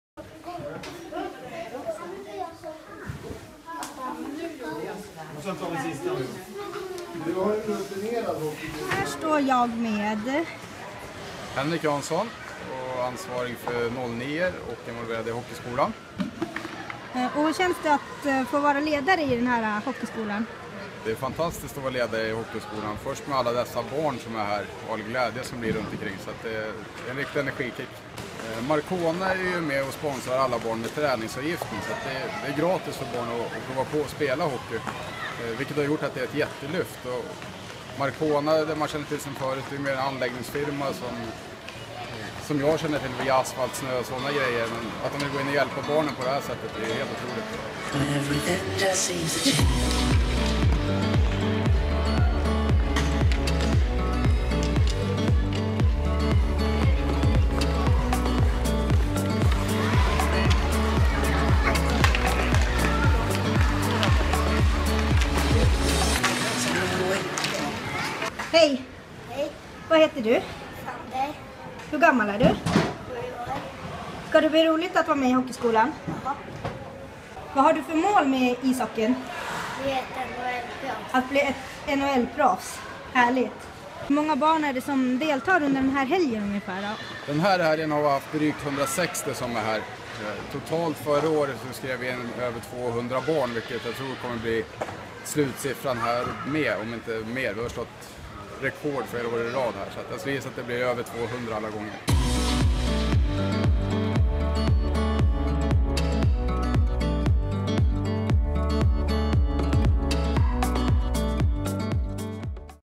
Uppstart av Hockeyskolan 2016!
Markona går in och sponsrar Almtunas hockeyskola i år igen, vilket möjliggör för hundratals barn att spela.